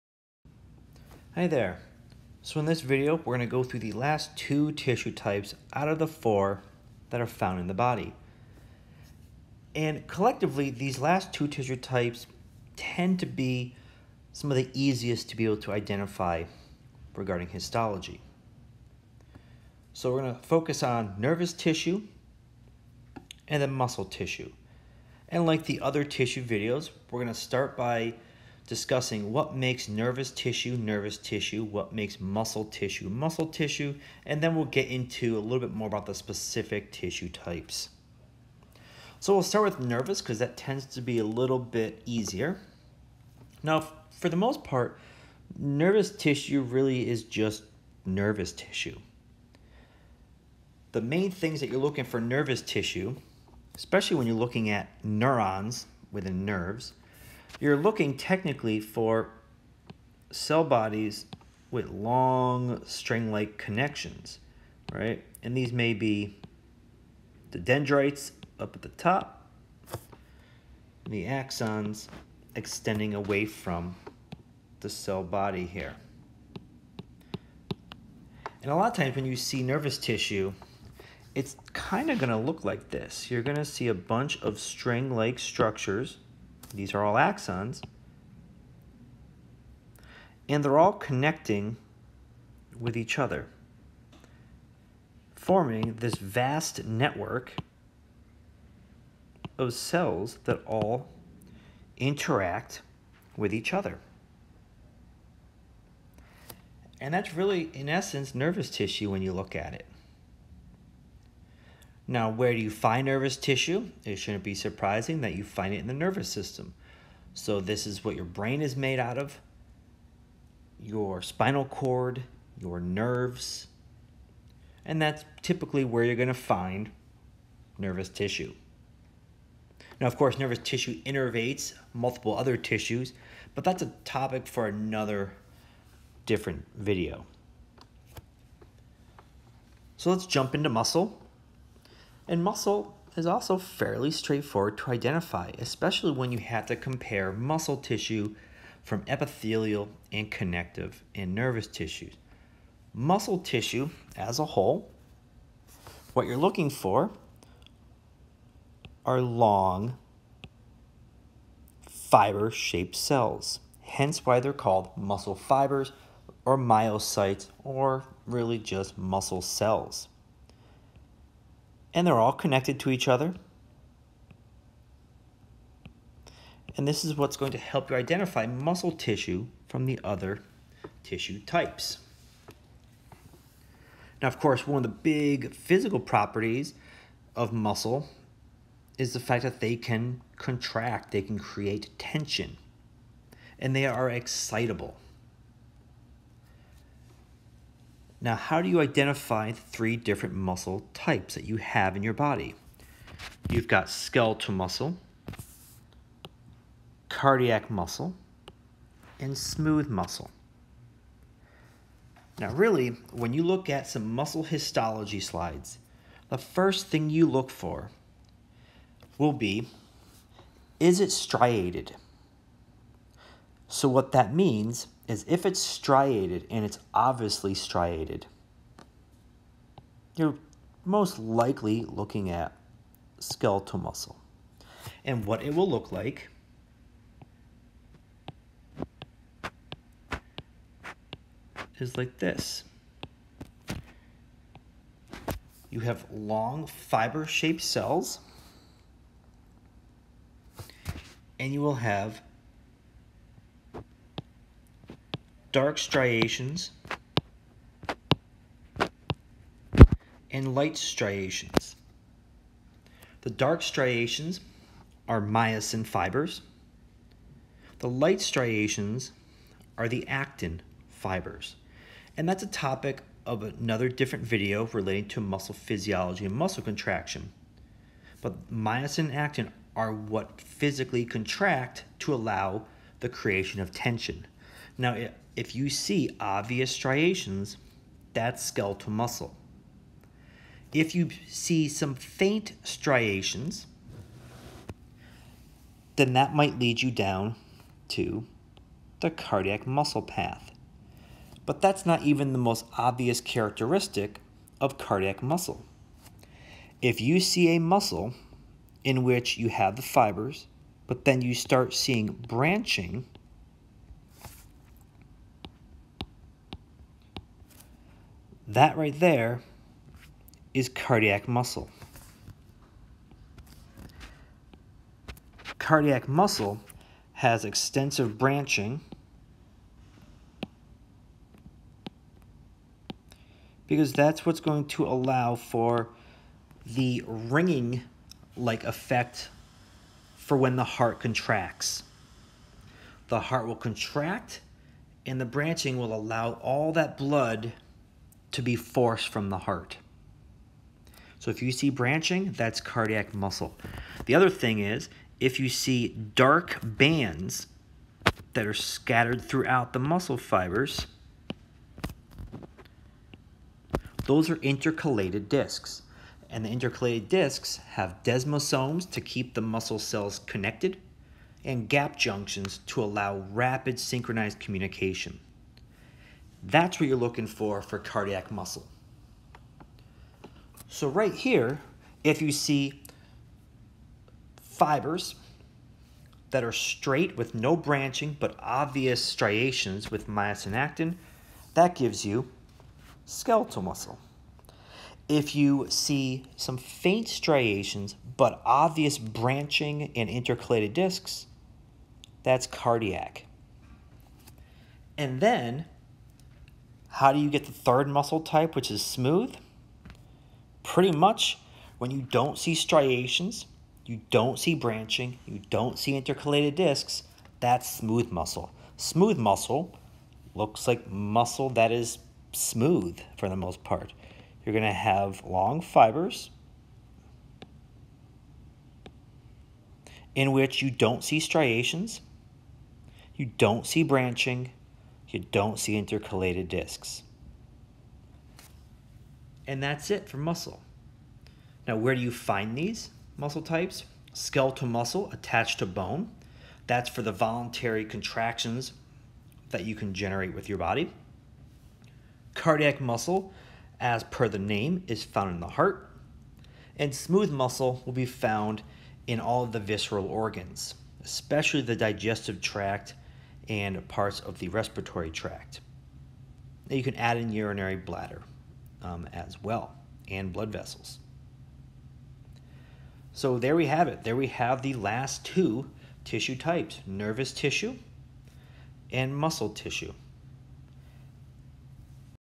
Muscle & Nervous Tissues
Details on muscle and nervous tissues and how to differentiate the different tissue types.

Created by Dr. Nicholas Pollock for A&P courses.